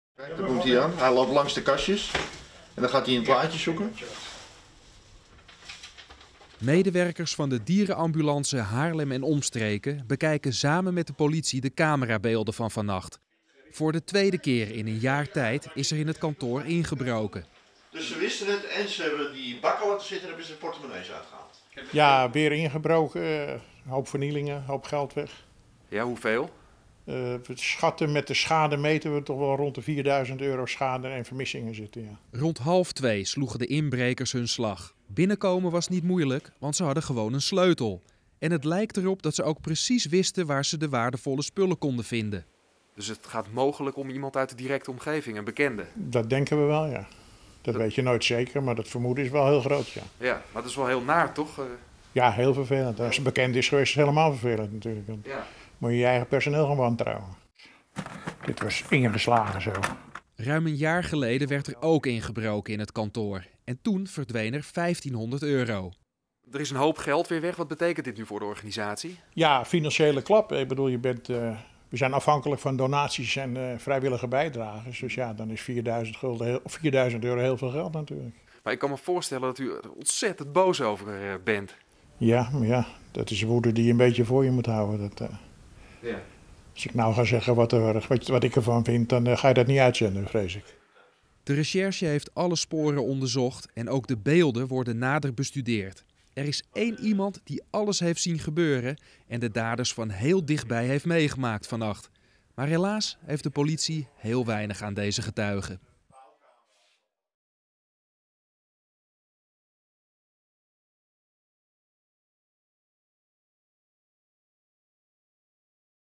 Weer inbraak bij Dierenambulance Haarlem: 4000 euro schade
Een kluis staat open, een kastdeur is flink beschadigd en de inhoud van een prullenbak ligt door het kantoor verspreid. Het kantoor van de Regionale Dierenambulance Haarlem en Omstreken is vannacht bezocht door inbrekers. Net als vorig jaar november is er ook nu weer een paar duizend euro verdwenen.